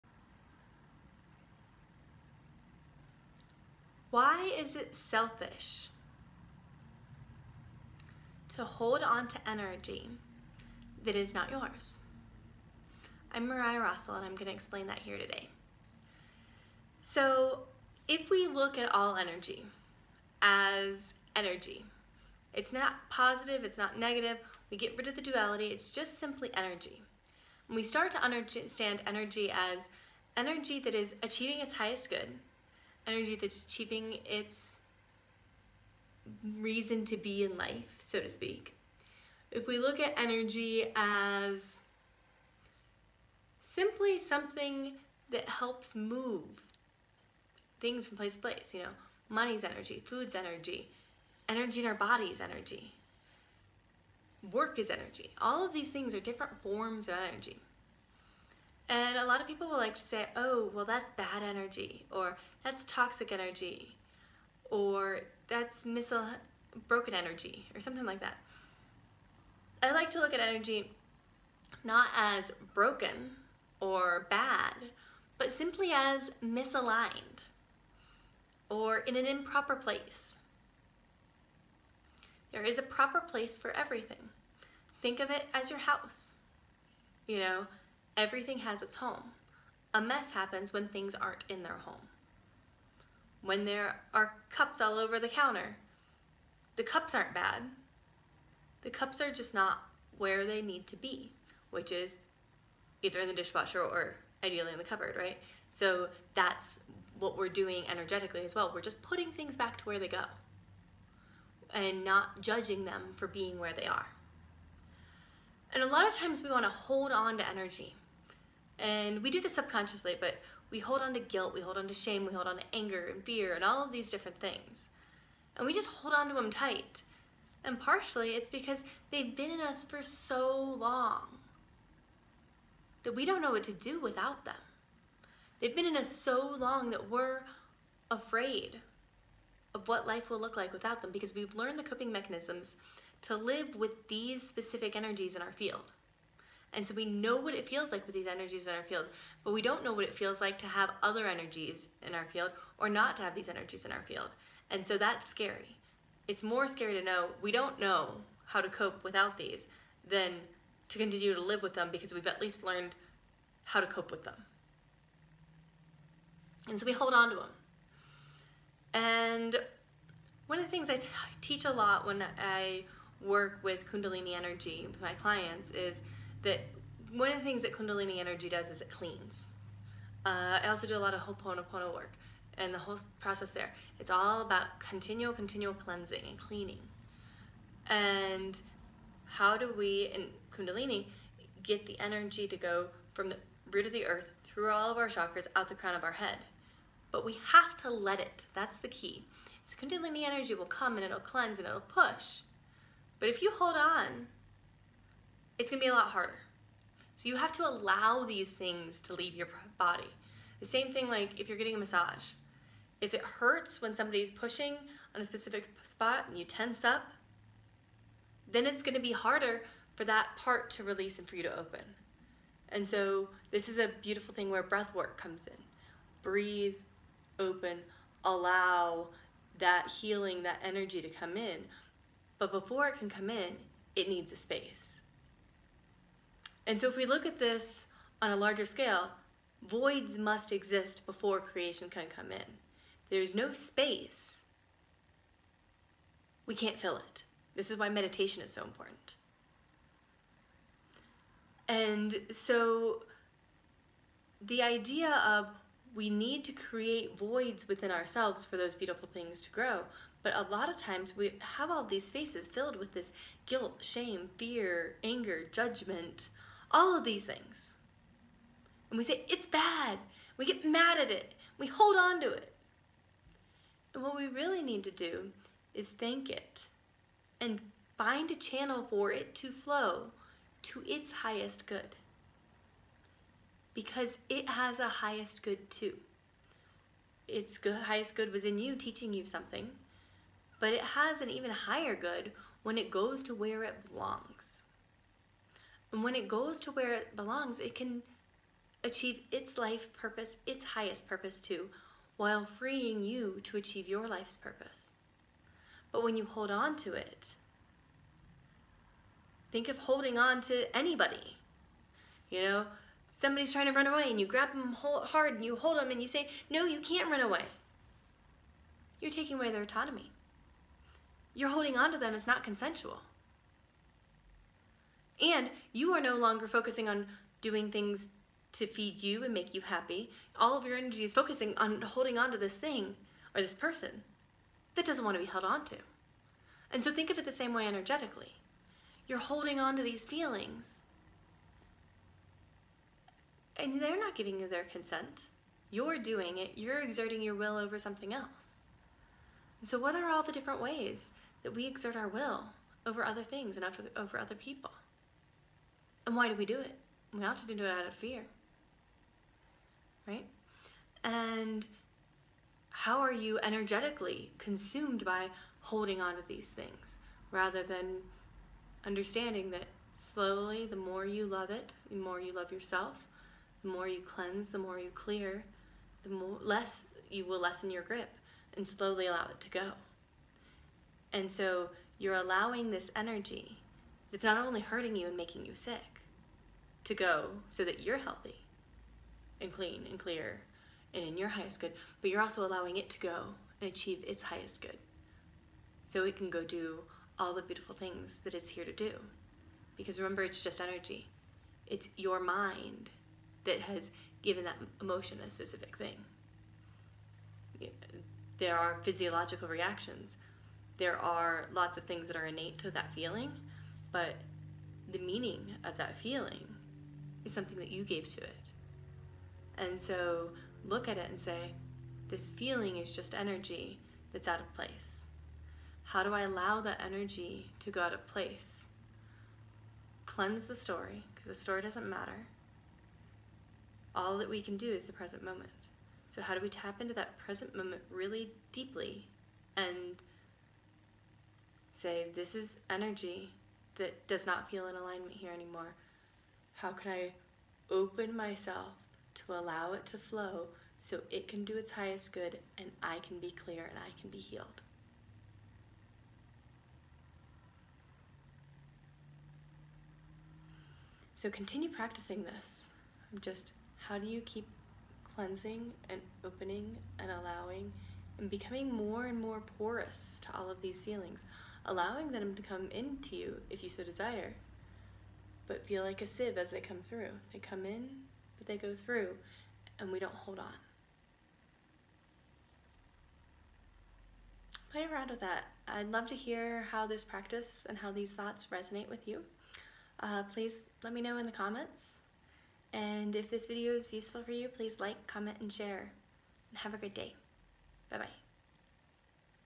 Why it is Selfish Not to let go of Misaligned Energy
In this video, I explore why letting go of the energies that make you feel stuck is in the highest good for all beings.